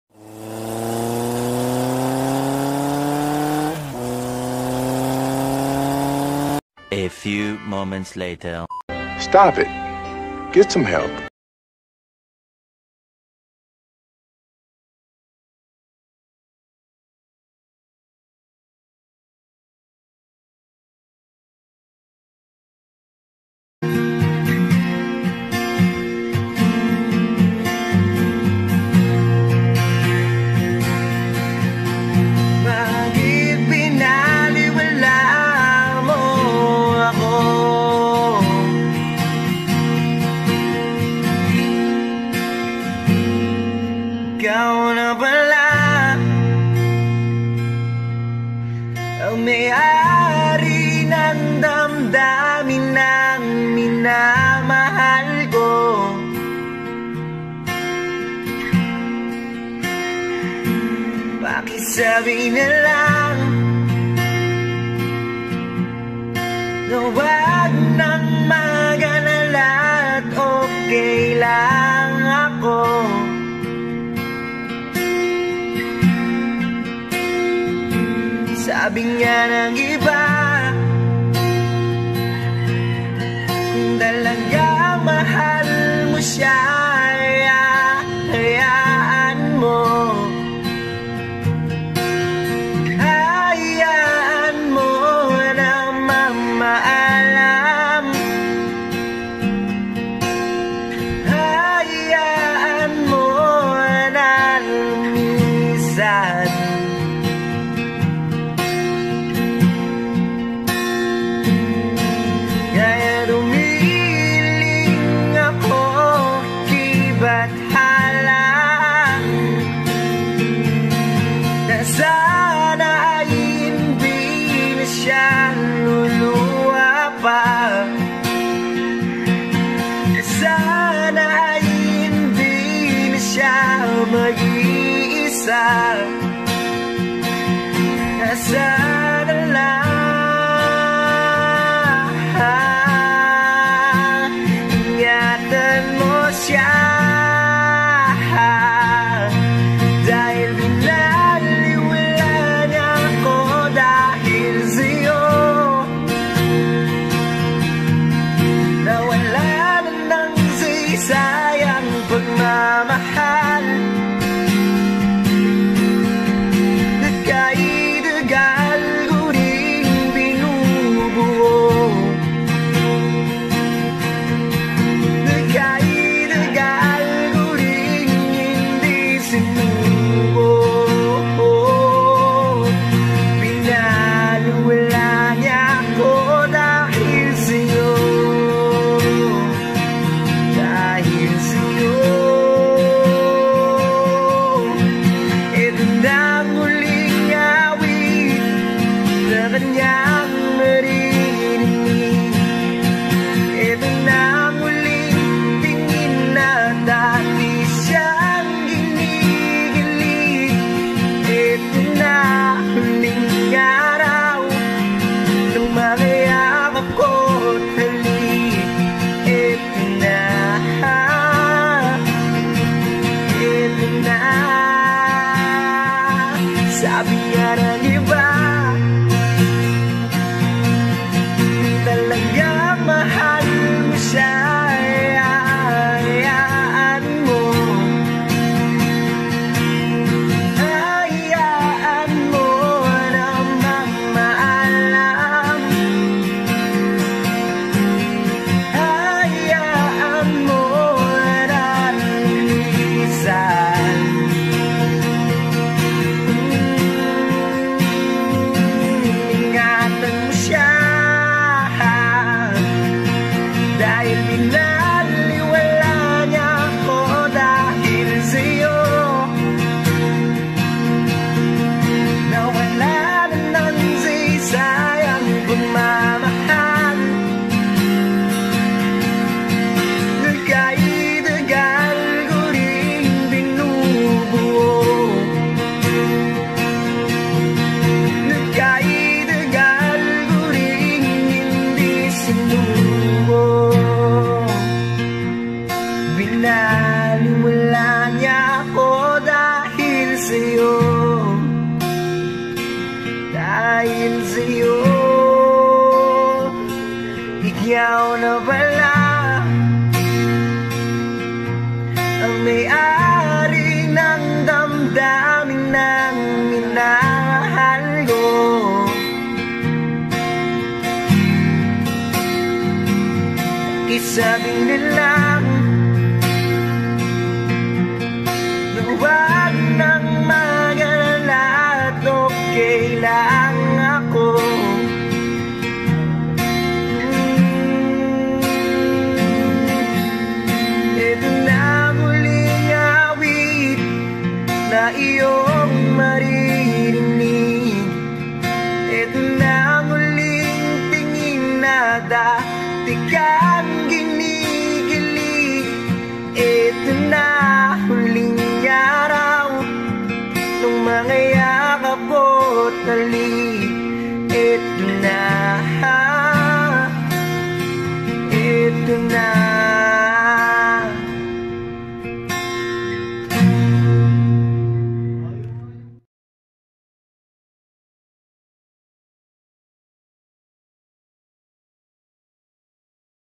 ESCAPE IN THE CITY
Takasan Muna natin Ang magulong syudad langhap Muna Tayo ng sariwang hangin Dito sa probinsya